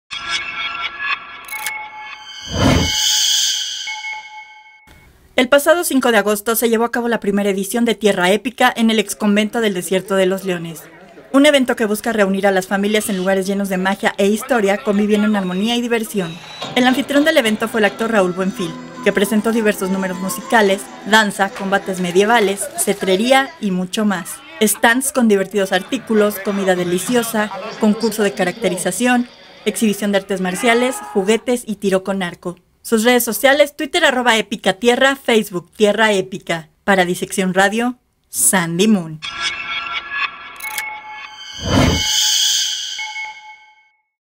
TIERRA ÉPICA
El pasado 5 de agosto se llevó a cabo la primera edición de Tierra Épica en el Ex Convento del Desierto de los Leones, un evento que busca reunir a las familias en lugares llenos de mágia e historia, conviviendo en armonía y diversión.